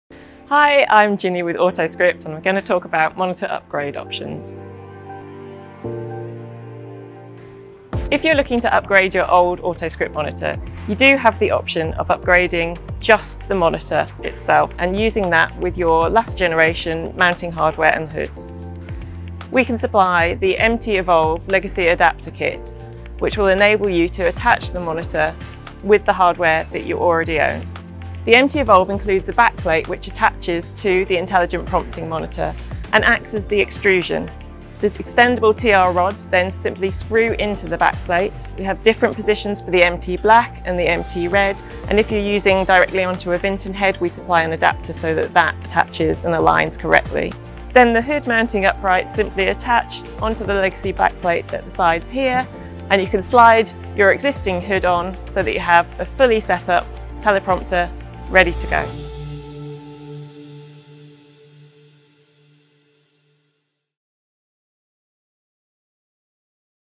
Upgrade to Intelligent Prompting
Upgrading previous generation Autoscript prompting systems to the latest Intelligent Prompting EVO-IP and EPIC-IP monitors using the MT Evolve Legacy Adapter Kit.
Step up to Intelligent Prompting without changing your on-camera mounts or hoods.